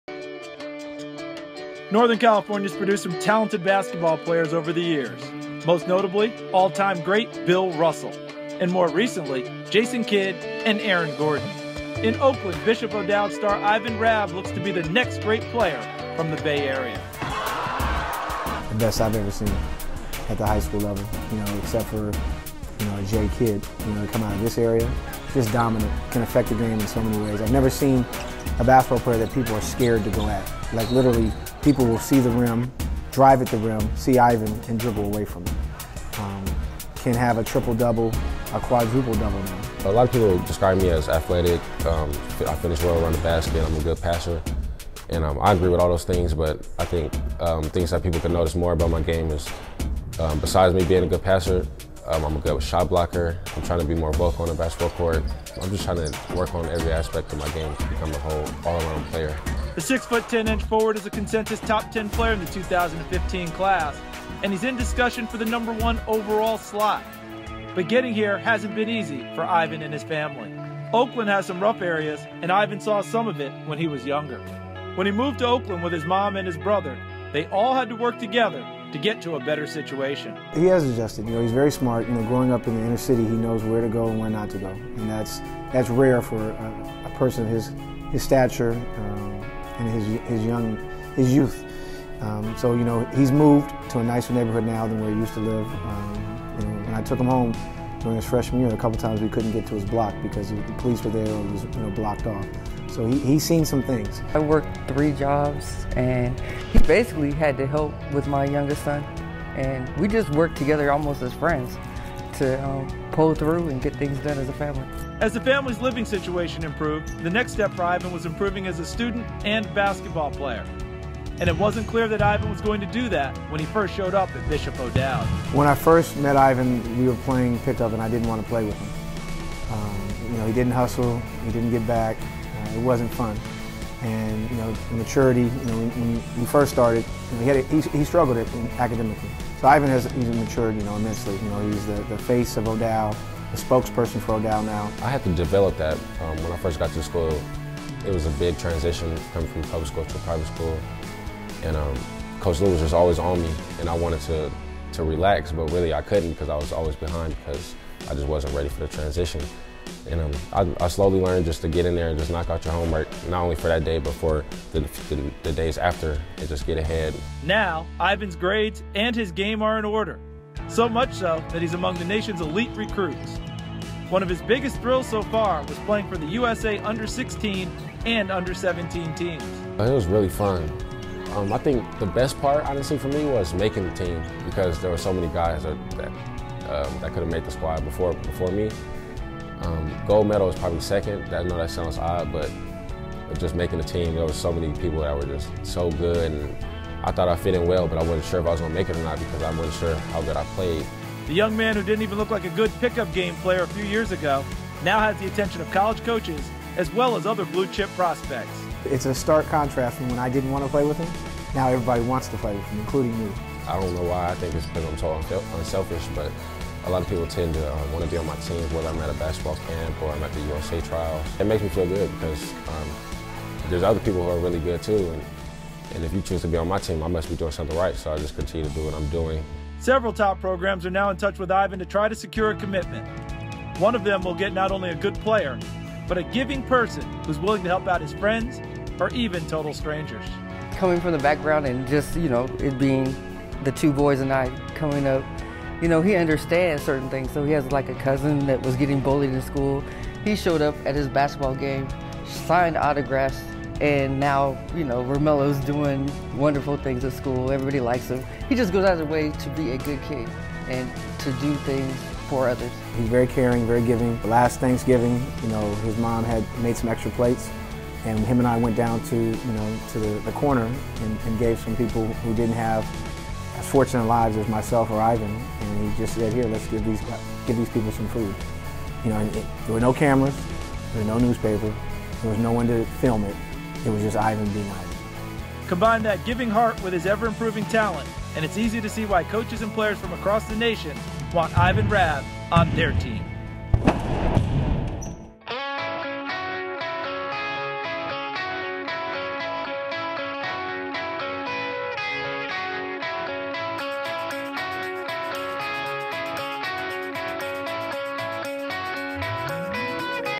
Ivan Rabb - Bishop O'Dowd Forward - Highlights/Interviews - Sports Stars of Tomorrow
Ivan Rabb is one of the most talented players in the 2015 class. The 6-10 forward at Oakland power Bishop O'Dowd hasn't had an easy road to the top, but his family and his coach have helped him succeed.